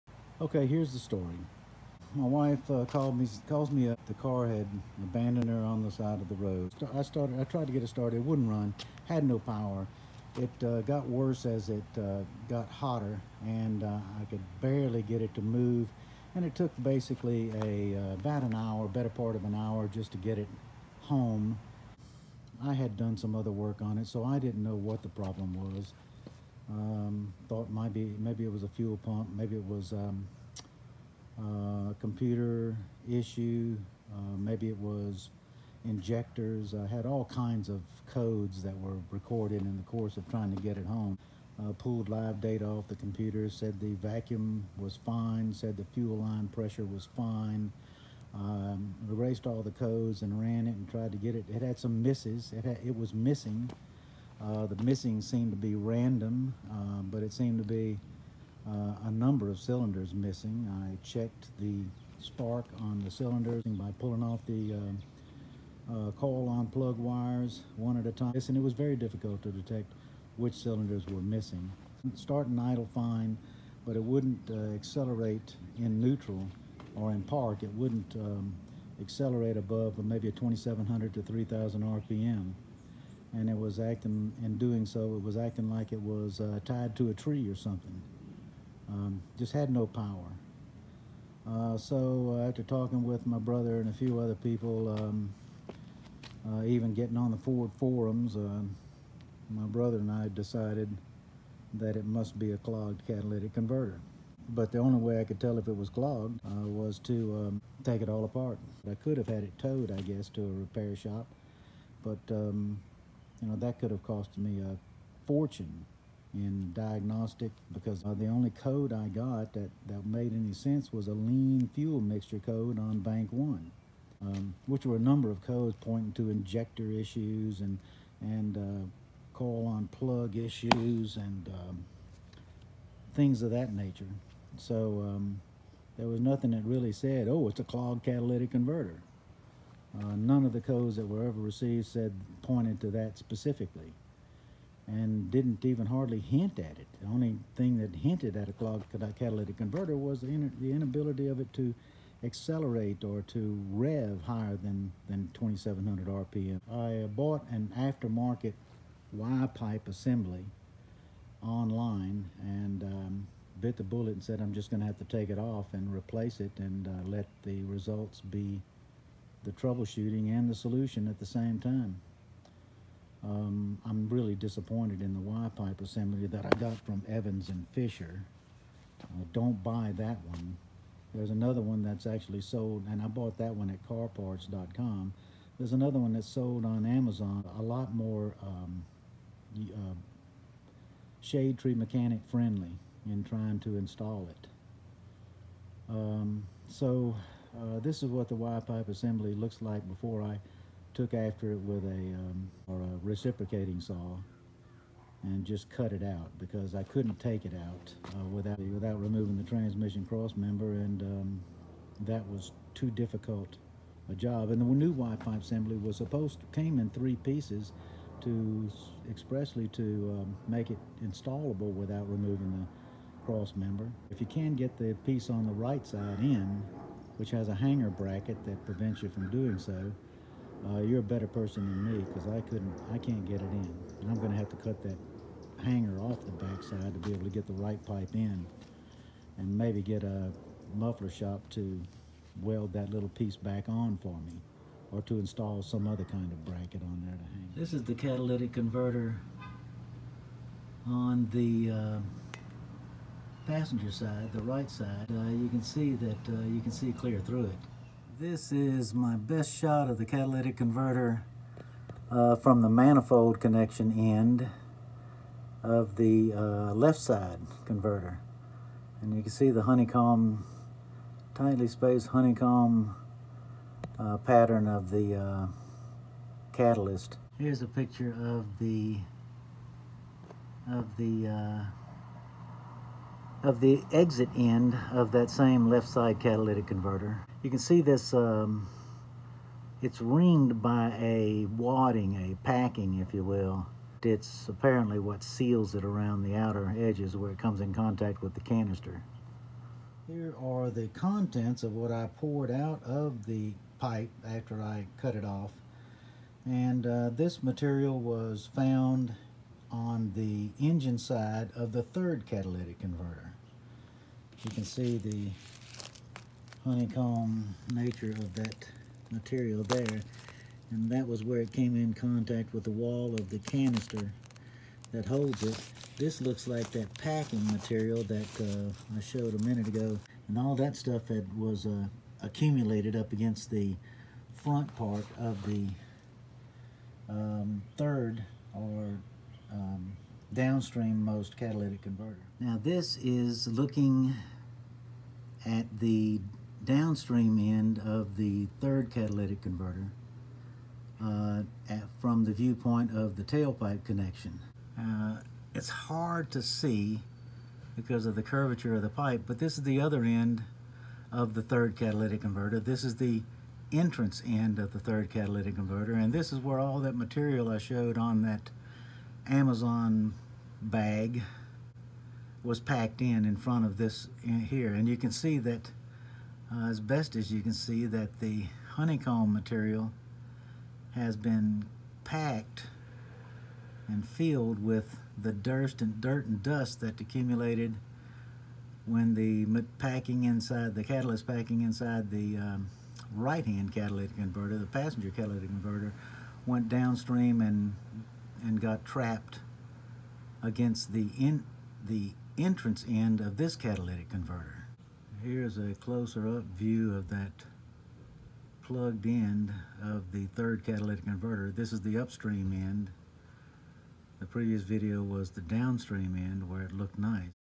2004 4.6 V8 Explorer clogged catalytic converter
Diagnosing a clogged catalytic converter.